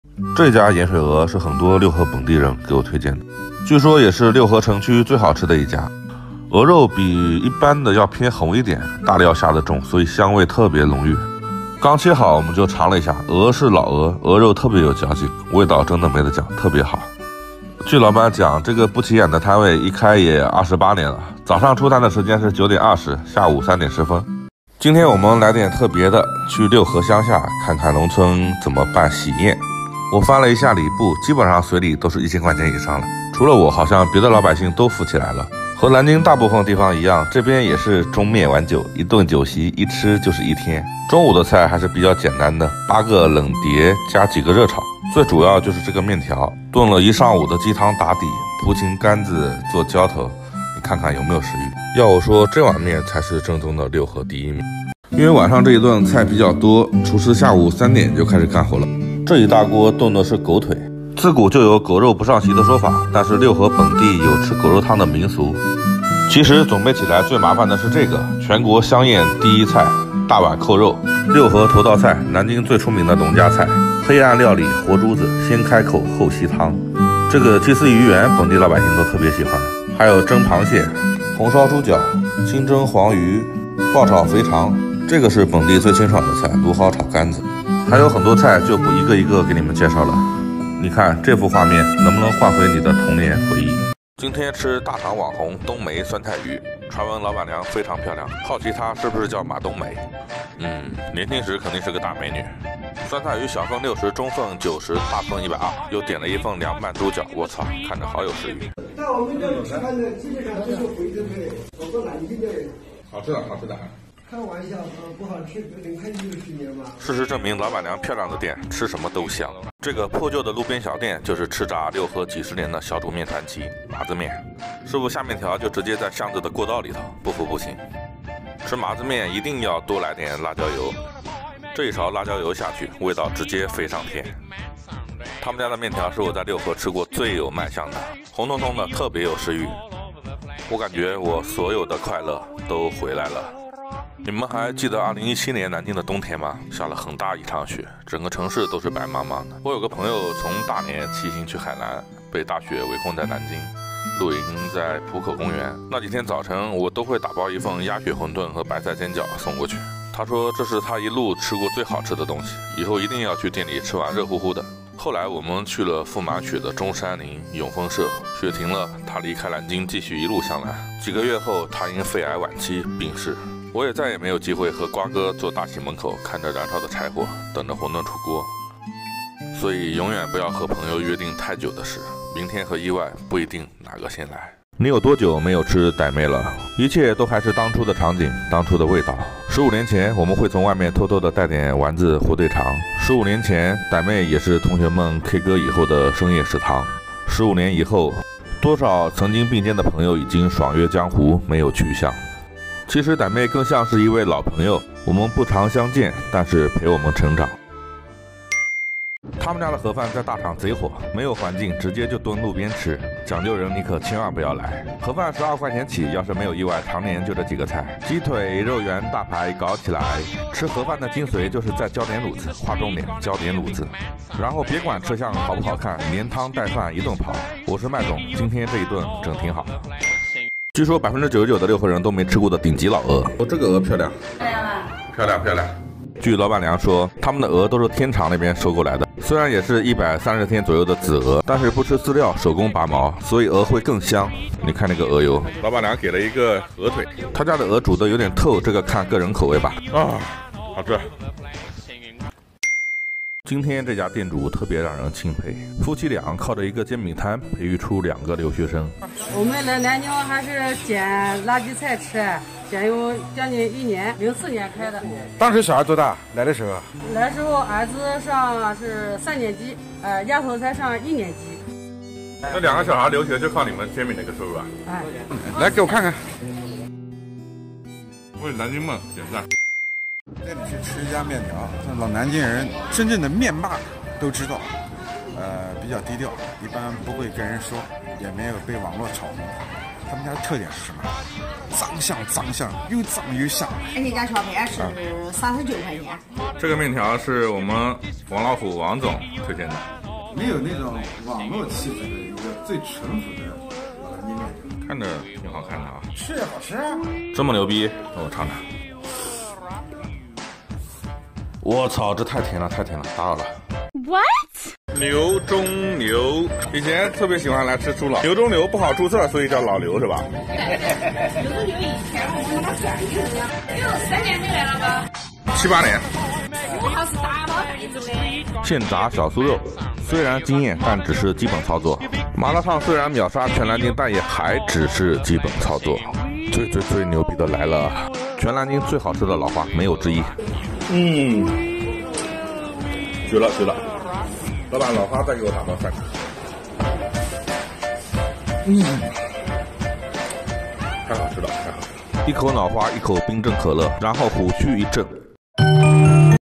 南京六合最好吃的12种美食！南京面条霸王！南京最好吃的麻辣烫！盒饭！
这个视频的目的是分享美食制作过程，与动物福祉无关，所有食材的获取均符合法律和道德要求！ 1. 0:00六合城区最好吃的盐水鹅，你吃过吗？南京美食
2. 0:24南京六合乡下的喜宴，一天要吃两顿，中午吃面条。南京
3. 0:55你还记得小时候吃酒席的画面吗？南京
4. 1:34我朋友信誓旦旦说这是大厂最好吃的酸菜鱼！没有之一！😂😂😂南京美食
5. 1:59西门麻子面，六合排前三有问题吗？😃评论区欢迎各位大侠们，友好的互喷！南京美食
6. 2:24剪辑完这段视频，眼泪已经包不住了。也想听听你关于美食的故事，哪家店，哪个人？南京美食
7. 3:04你有多久没有吃傣妹火锅了？南京美食
8. 3:29这家盒饭真的太好吃了！！！
9. 3:50据说99%的六合人都没吃过的老鹅，香的一比！南京美食
10. 4:13是不是每个城市都有摊煎饼买好几套房的故事？美食趣味计划
11. 4:40城南面条太甜，还是更喜欢六合的味精腰肚面！😂南京美食
12. 5:14就算骂我是托！我也要大声的说！这家是我眼里，南京最好吃的麻辣烫！南京美食
大家好，我是麦总，生活中一大爱好是尝遍美食